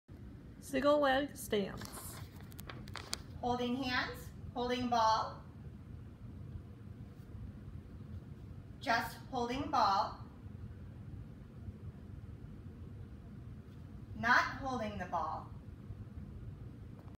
Staggered standing on a ball
Hand held assist with ball stabilized
No hand held assist with ball stabilized
No hand held assist without ball stabilized